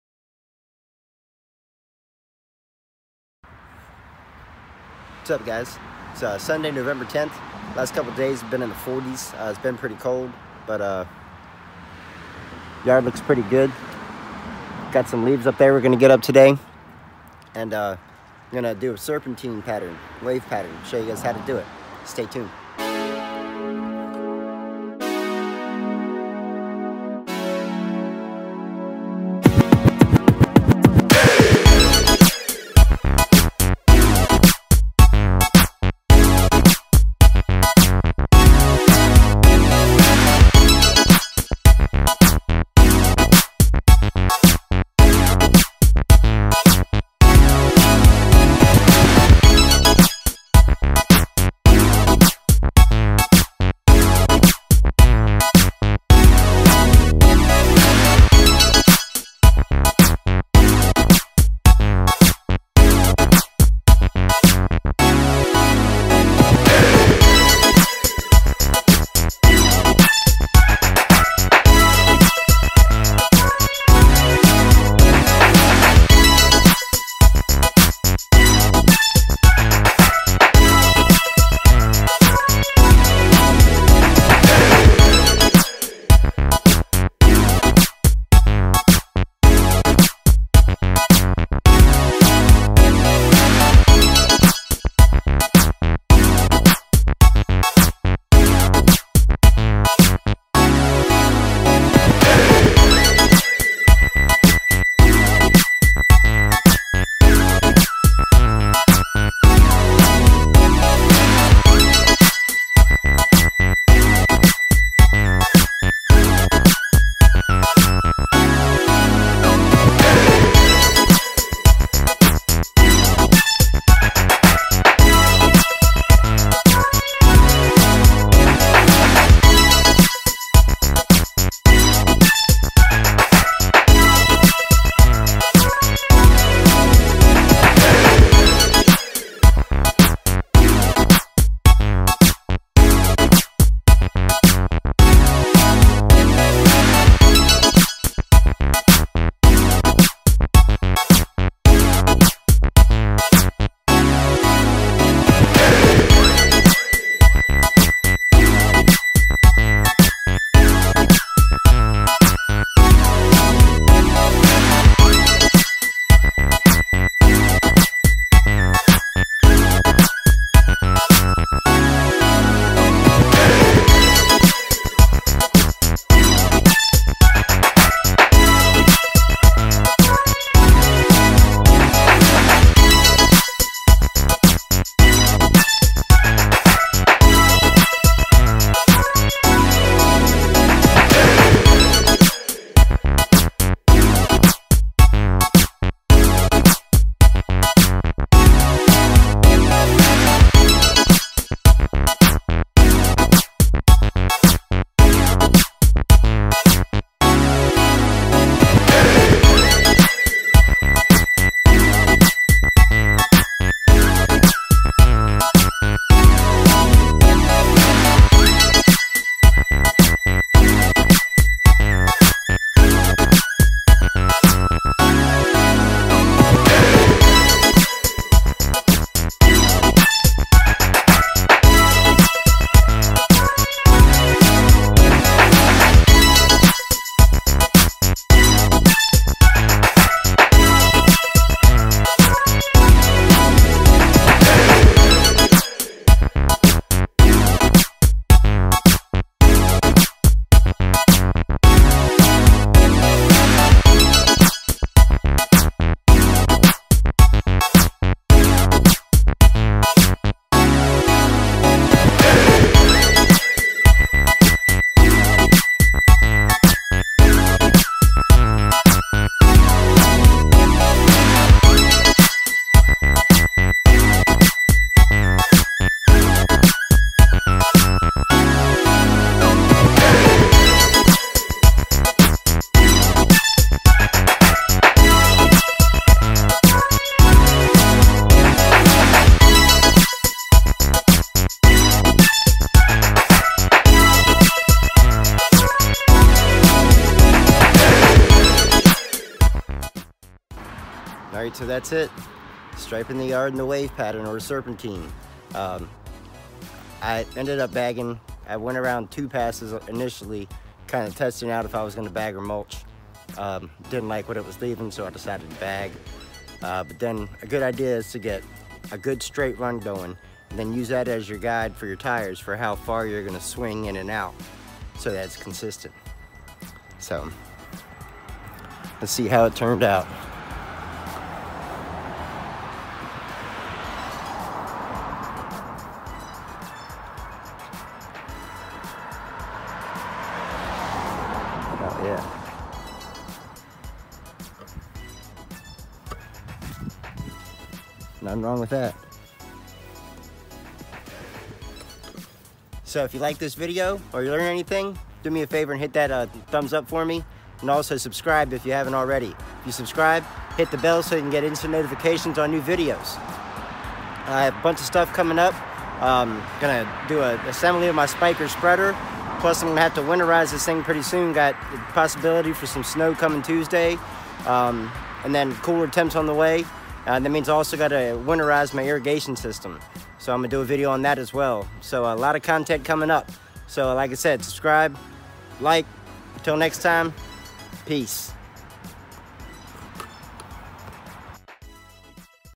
MOWING LAWN STRIPES // Serpentine stripes // wave pattern
Mowing lawn stripes. Serpentine stripes, or the wave pattern, is an attractive way to stripe the lawn. Lawn stripes, particularly curved lawn stripes, really catch the eye. Fall lawn stripes are so easy to do because the turf is healthy and thriving this time of year! Lawn striping in fall can also be done while removing leaves! The ultimate stripe can be achieved using a stripe kit, such as my checkmate stripe kit made by Big League Lawns.

You can purchase the products I use on my lawn here!